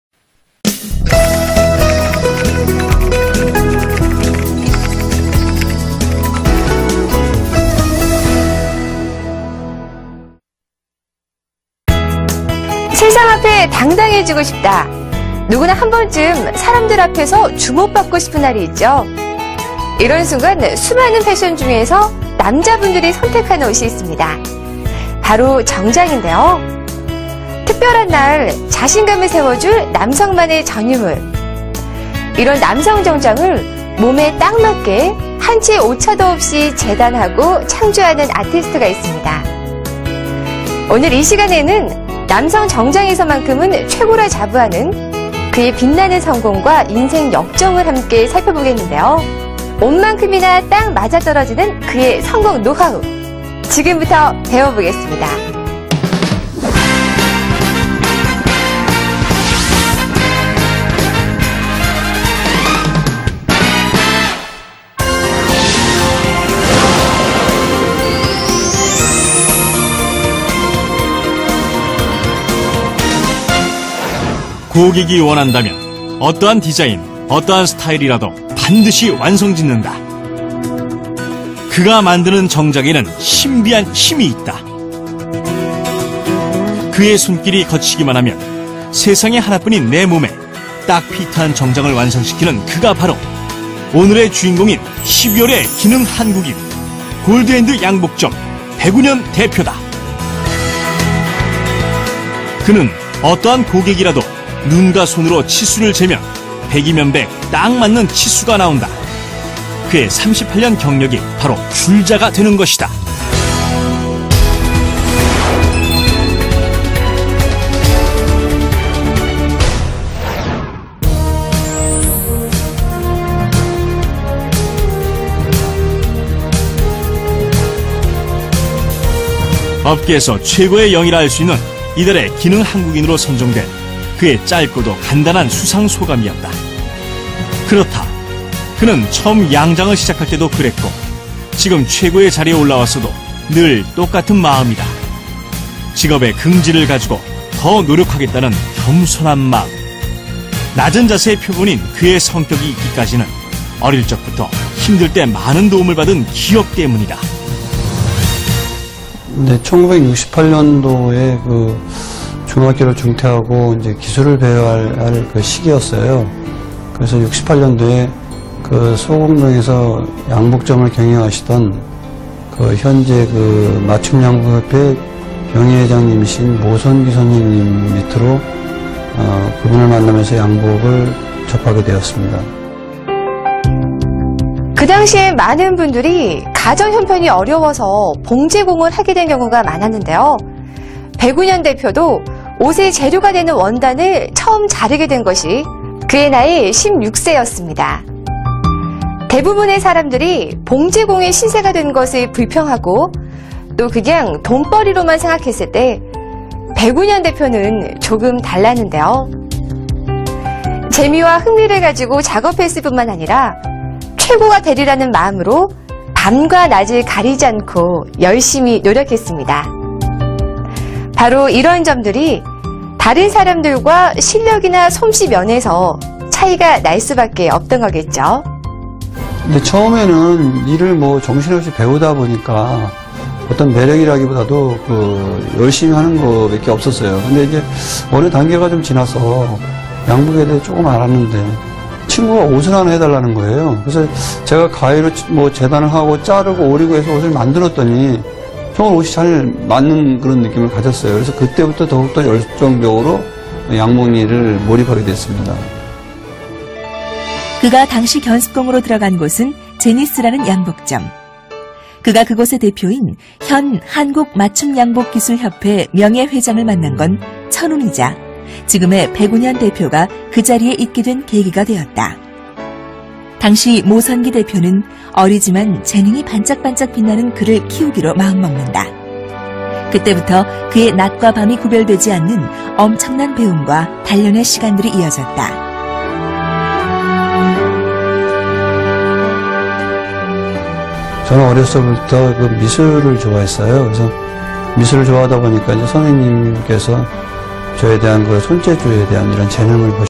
대한민국을 빛낸 기능한국인(2007년 12월 기능한국인 "백운현")
* 기능한국인 17호 백운현이 출연하여 자신의 성공사례를 안내함